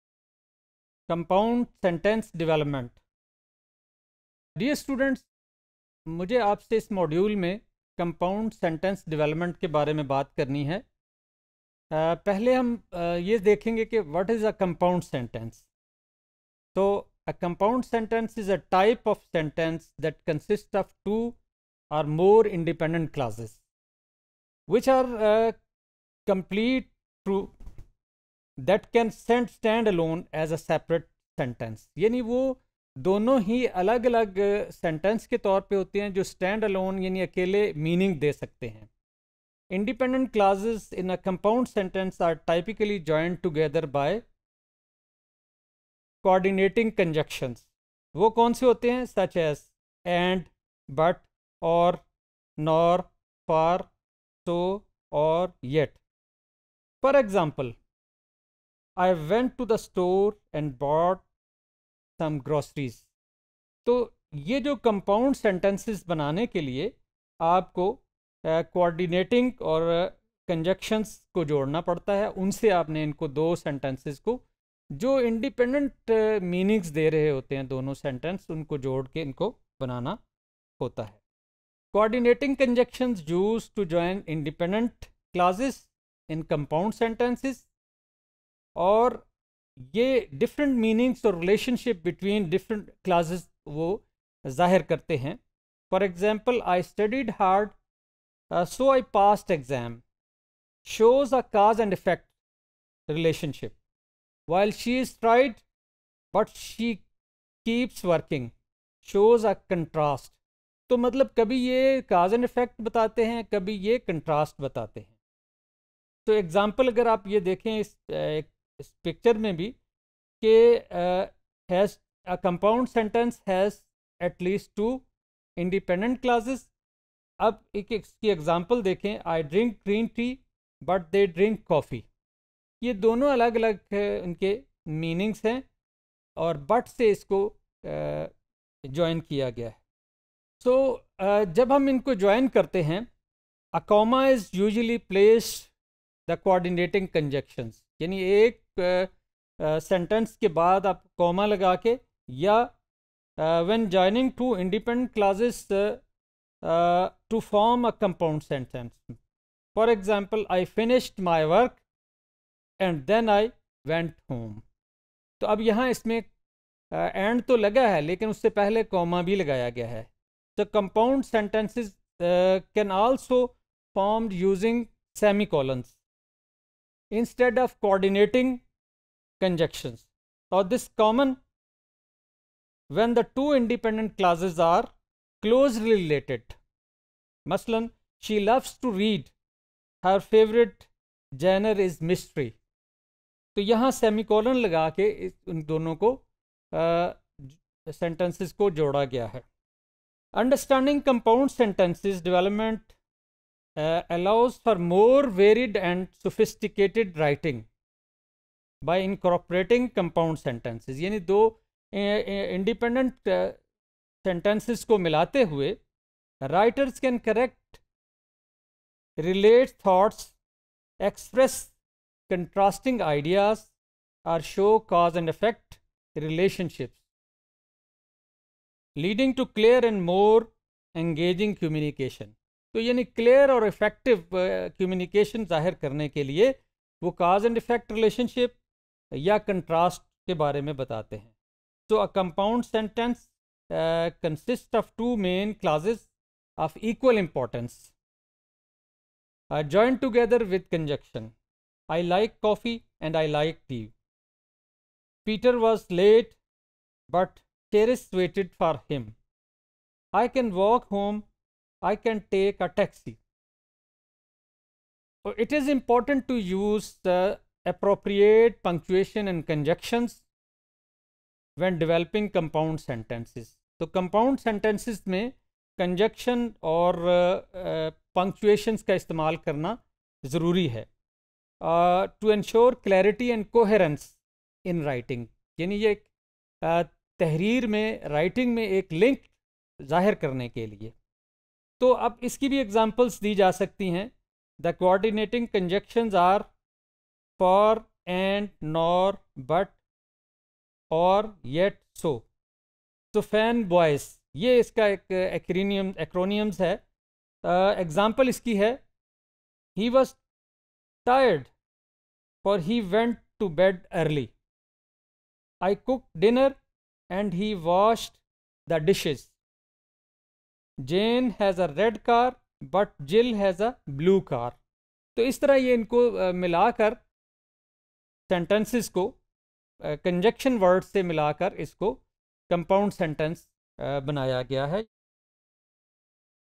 Compound Sentence Development | Language Development of the Child | ECE201_Topic112
Topic112: Compound Sentence Development,
By Dr. Muhammad Iqbal